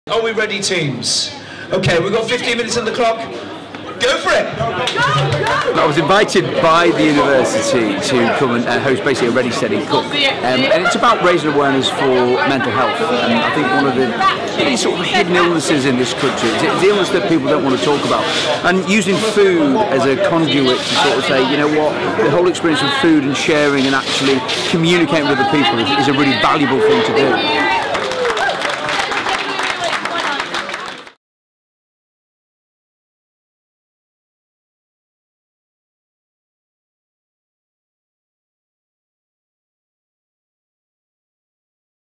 Simon Rimmer at DMU for University Mental Health Day
In recognition of University Mental Health Day, Chartwells held a special cook-off this Wednesday hosted by celebrity chef and DMU graduate Simon Rimmer!